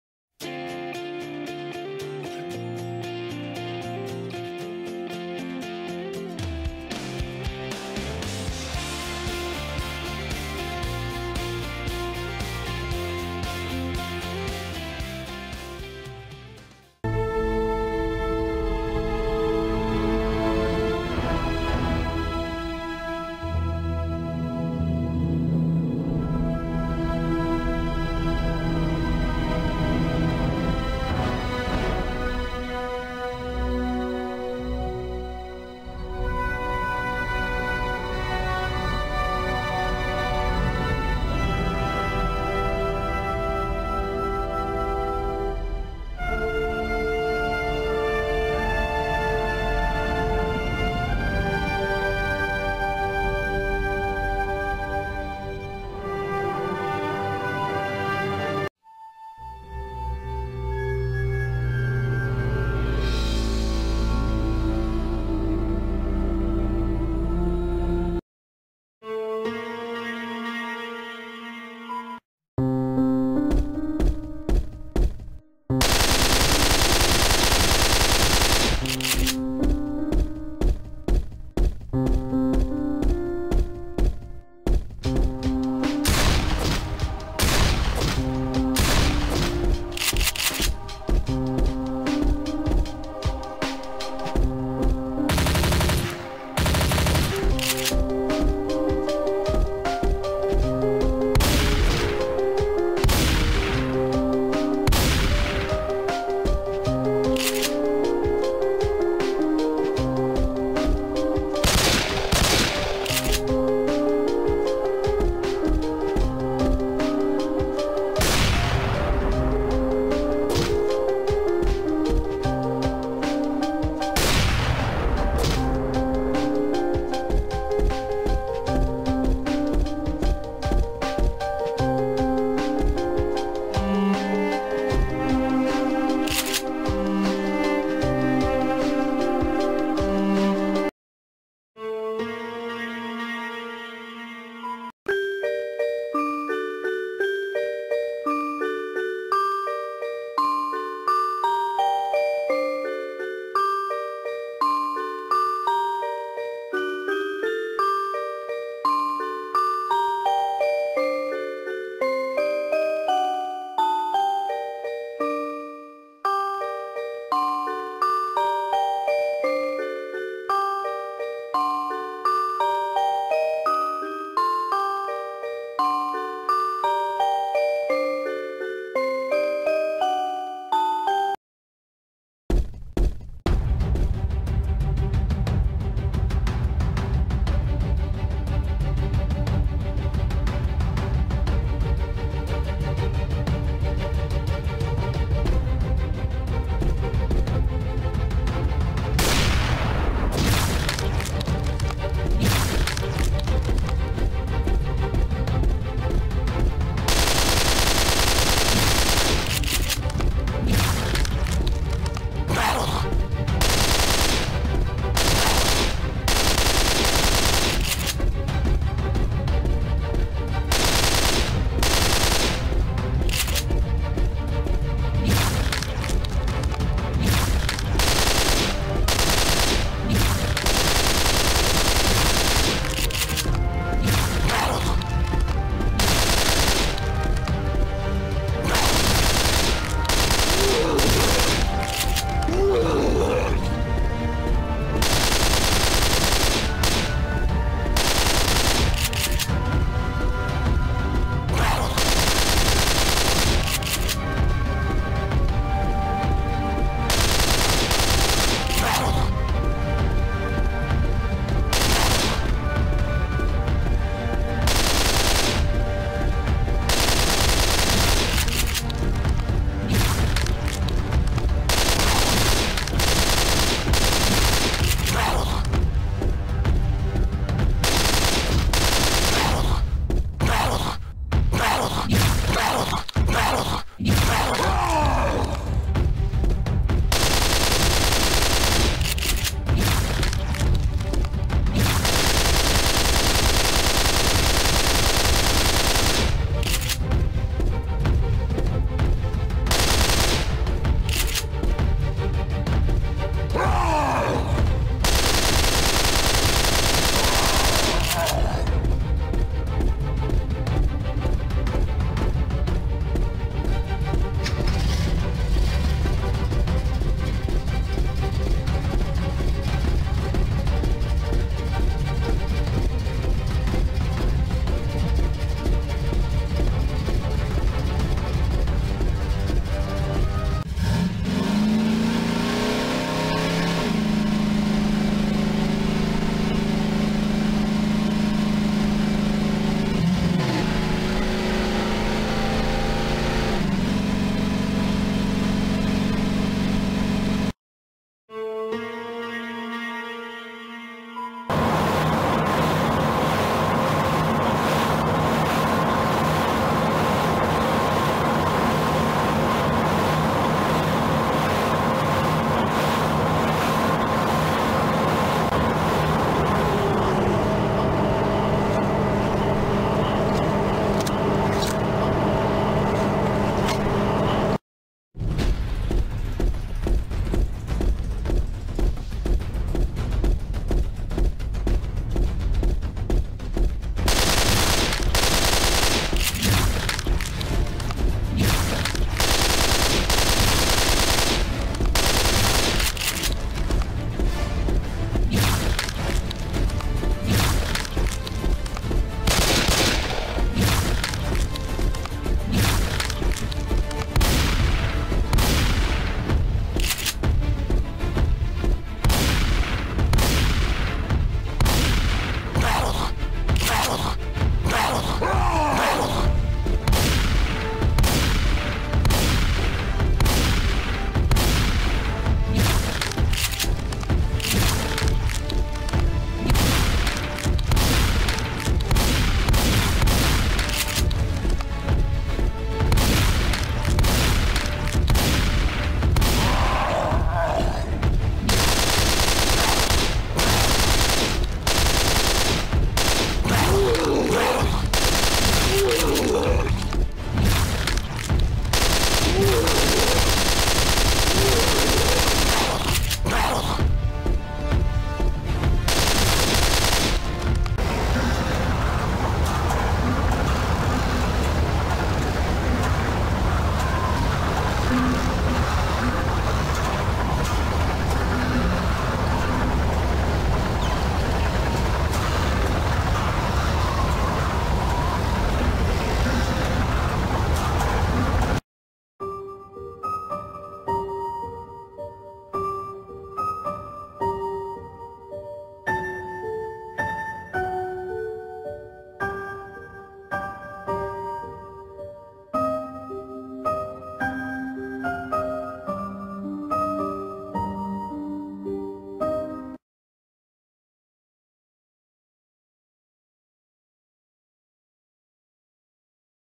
[Left 4 Doom 모작]쥬신게임아카데미 게임프로그래밍학원 6개월차 SR 팀 시연회_94기 14조 작품_김용하 신상훈 이의현 이현_클라이언트
쥬신게임아카데미 게임프로그래밍학원 6개월차 SR 팀 시연회_94기 14조 작품_김용하 신상훈 이의현 이현_클라이언트